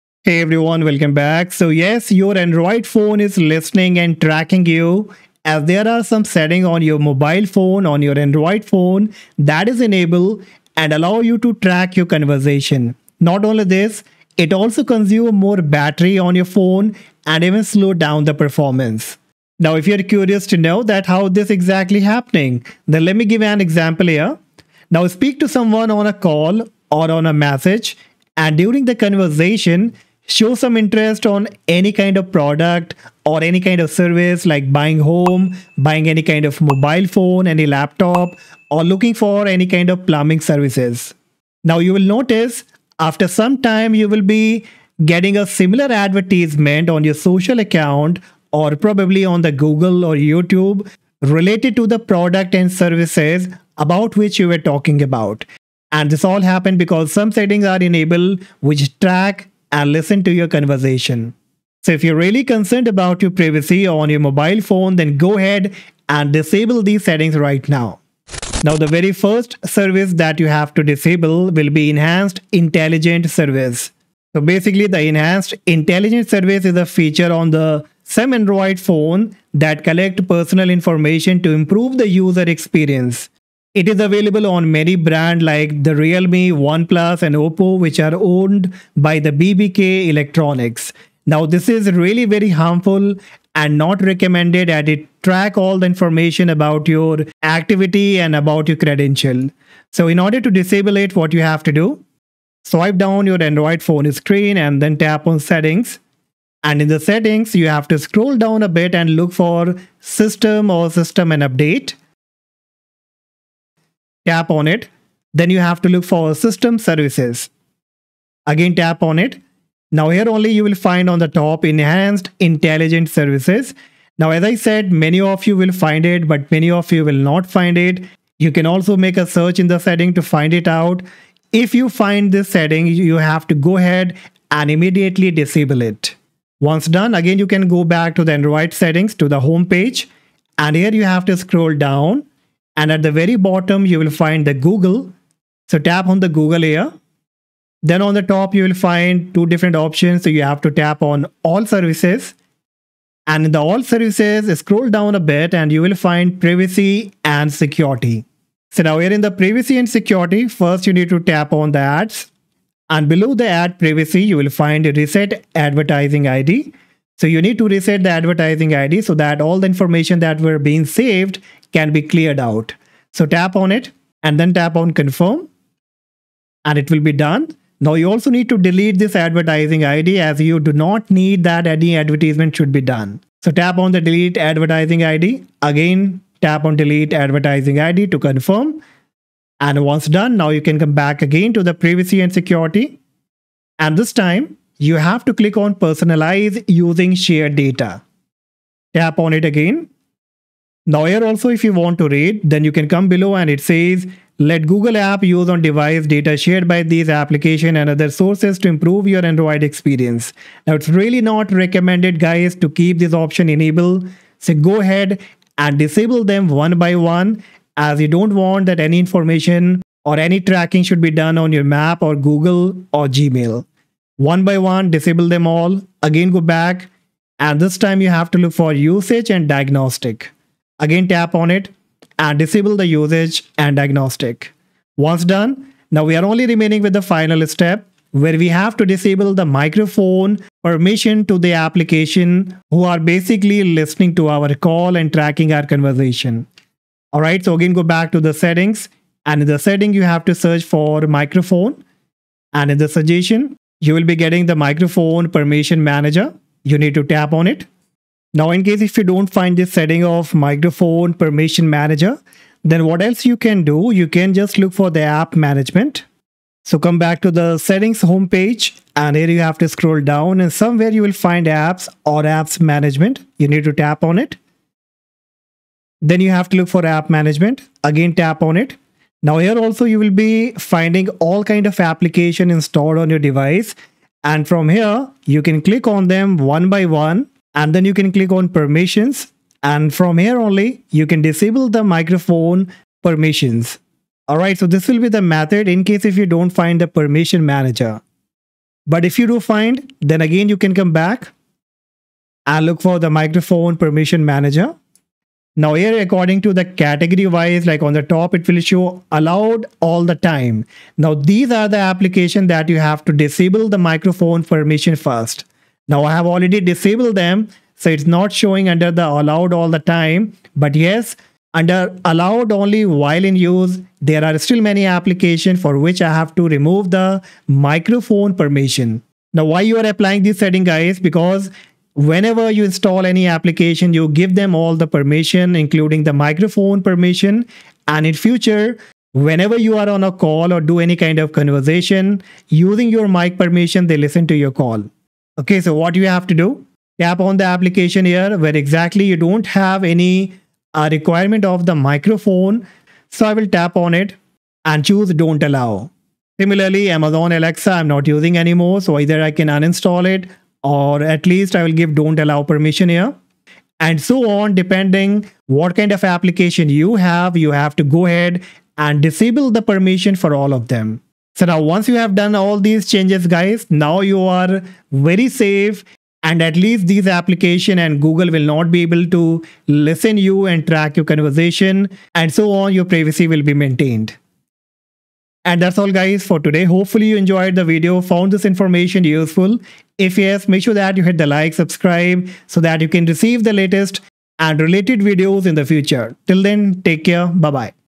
Your ANDROID Phone is TRACKING & LISTENING You - TURN it OFF
There are a few settings on your Android phone that are enabled and allow many apps to listen to and track your conversation on a call or either through text message.
If you are really concerned towards your privacy then you must disable these settings right now.

☑ Watched the video!
☐ Liked?